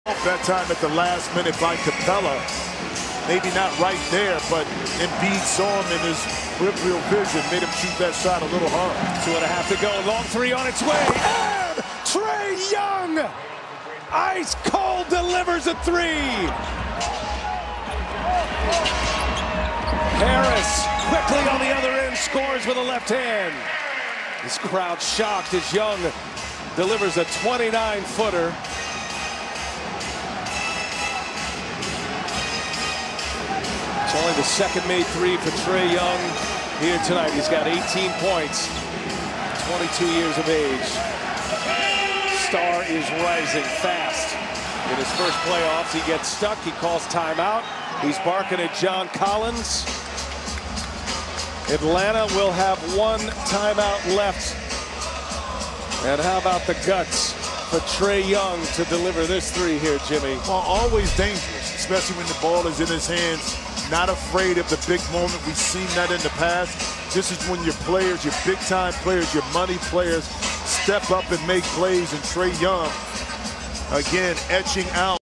Trae Young Hits a CLUTCH THREE From Deep, Silences the 76ers Crowd
Atlanta Hawks vs Philadelphia 76ers - Full Game 7 Highlights | June 20, 2021 | 2021 NBA Playoffs